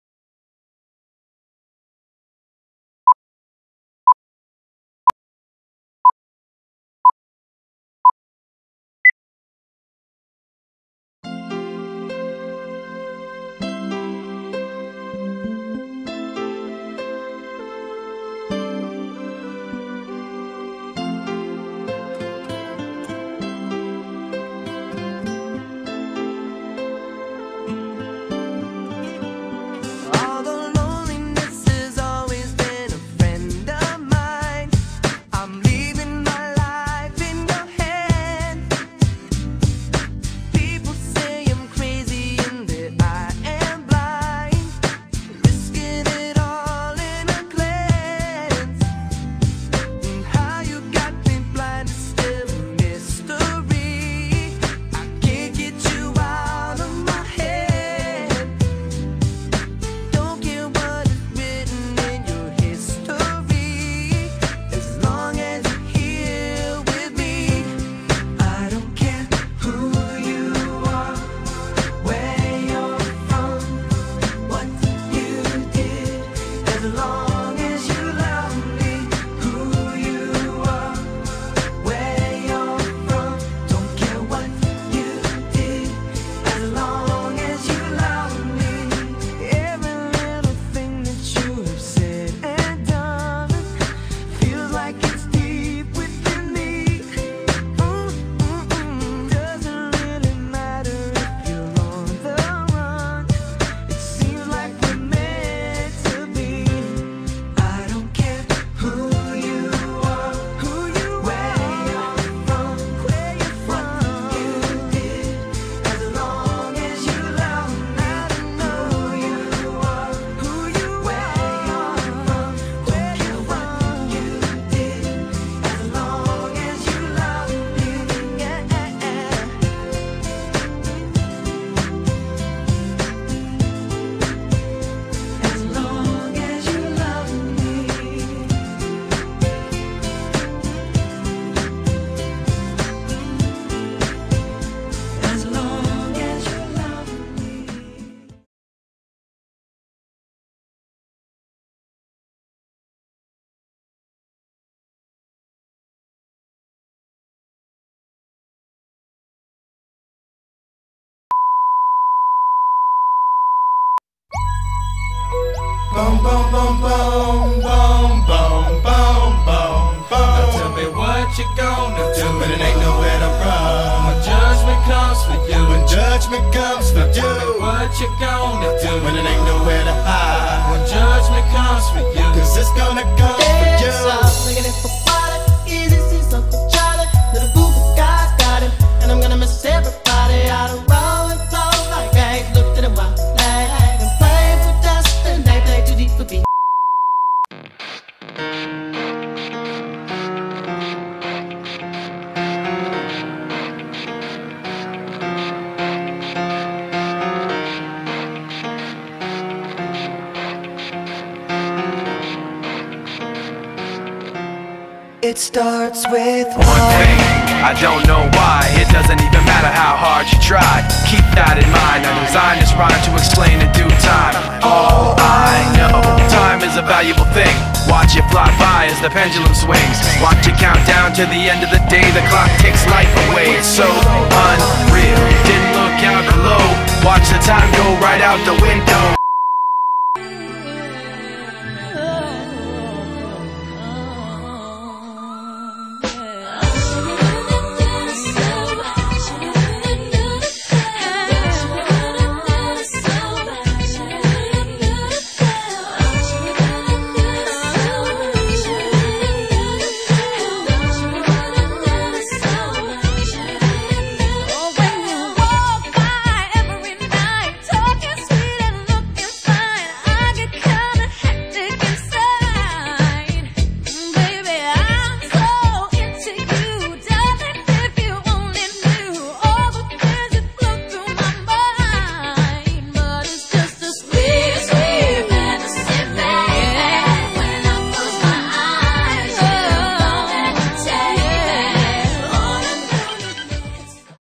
A song for Nancy and Adam Wang's wedding
these groomsmen wanted to showcase their musical talents. They couldnt figure out what songs Adam would like. Stay and watch the bloopers